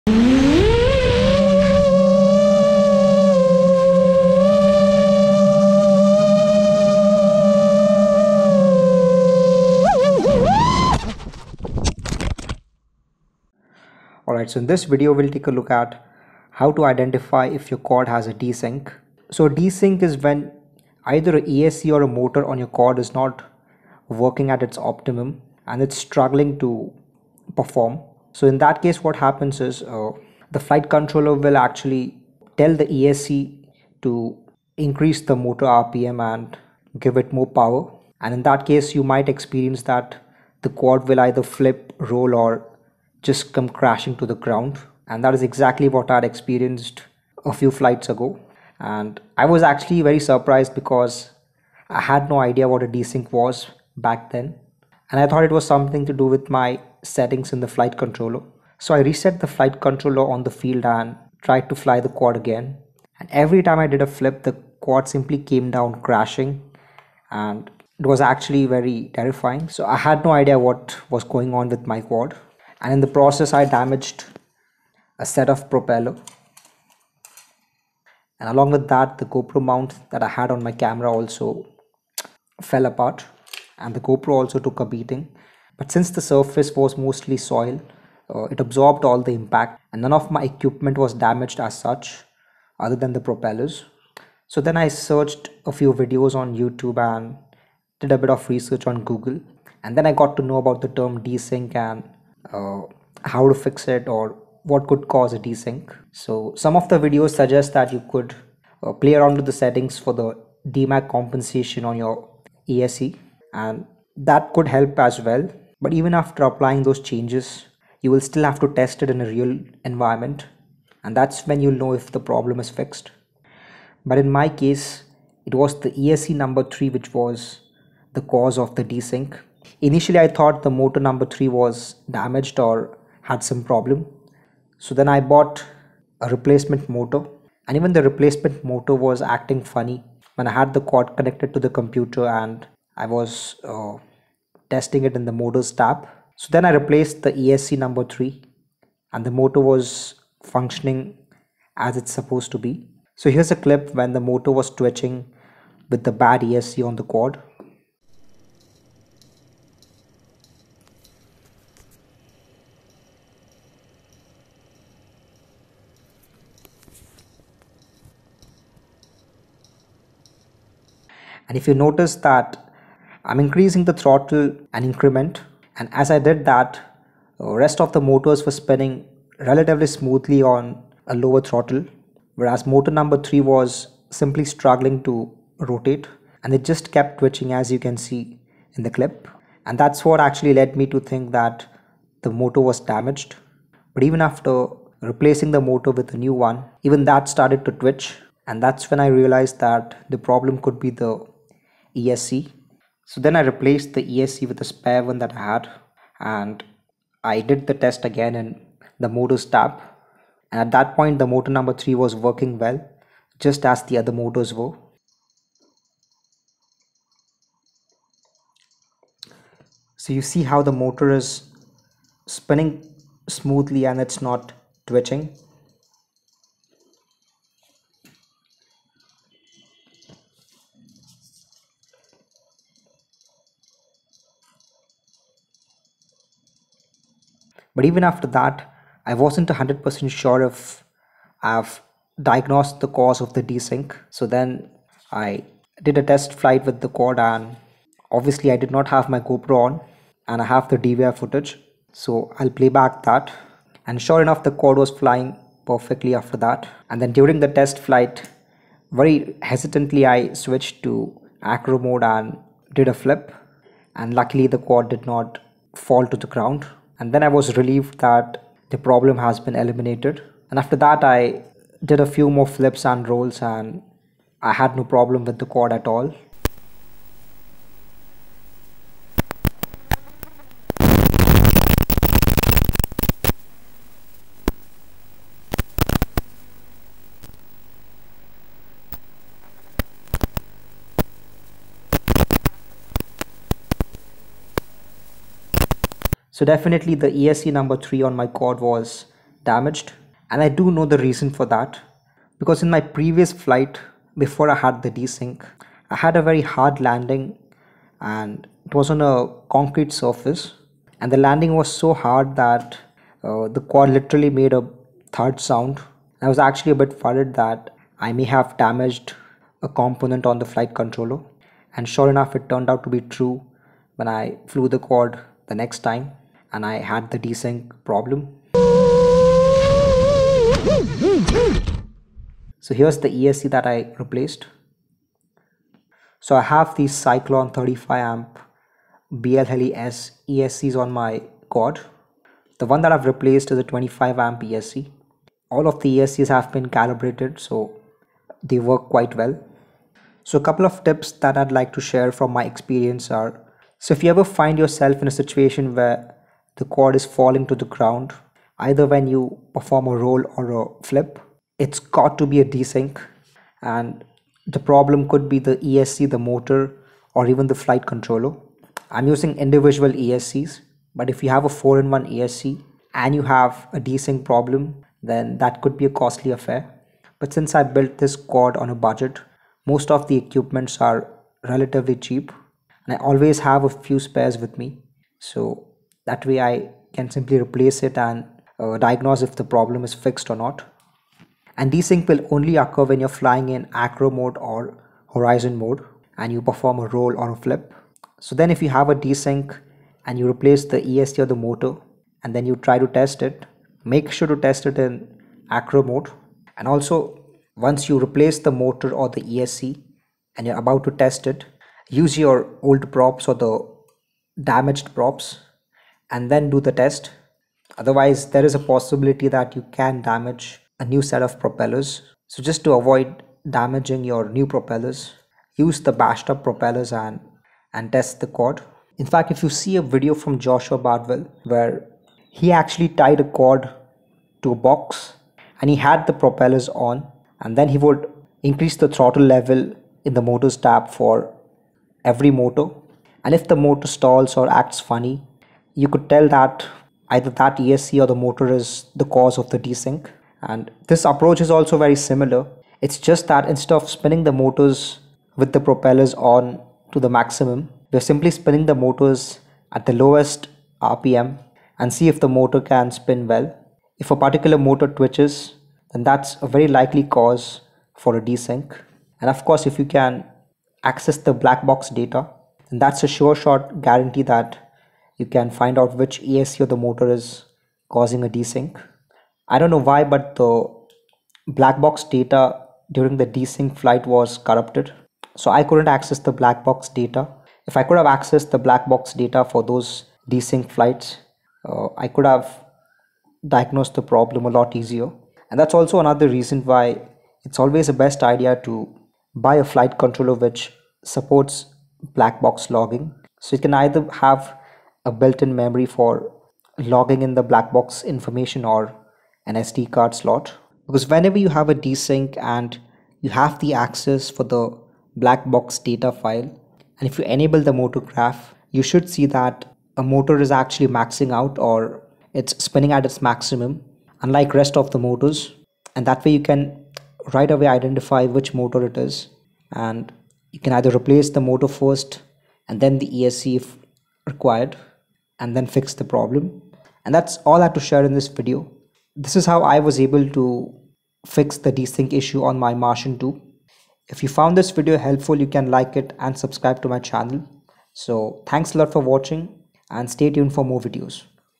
Fix FPV Quad Motor Twitching & DeSync Death Roll Falling to Ground
In this video i share how to diagnose a bad motor or esc on your fpv drone / quadcopter which could cause a desync leading to the quad falling out of the sky also referred as flip or roll of death.